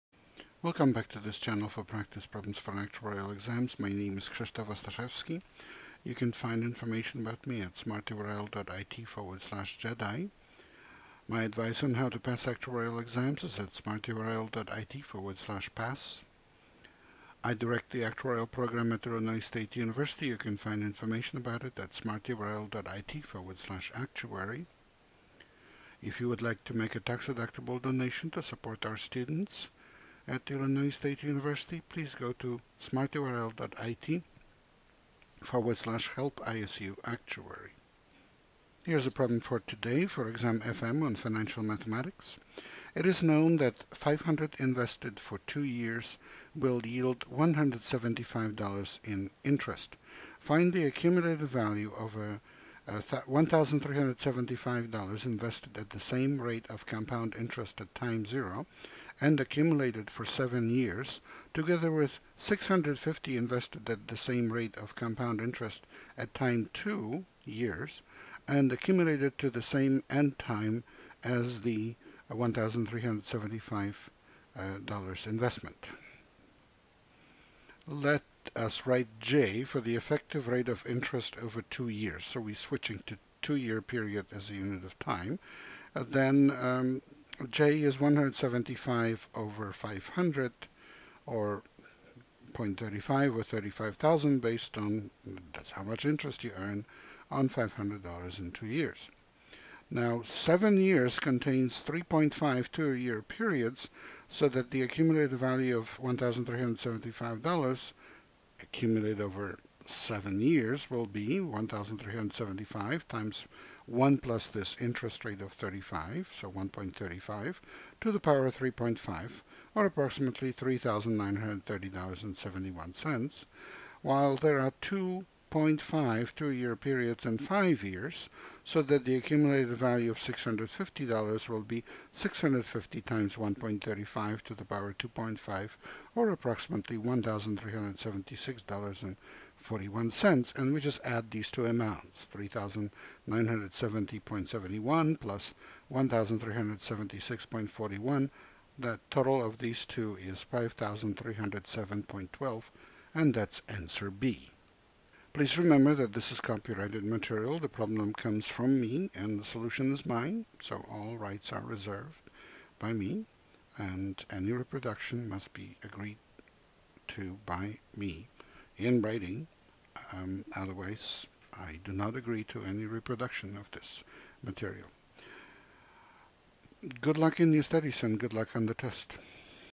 Exam FM exercise for May 28, 2018
Exam FM exercise for May 28, 2018, Dr. Ostaszewski’s online exam FM exercise No. 13 posted August 13, 2005, video solution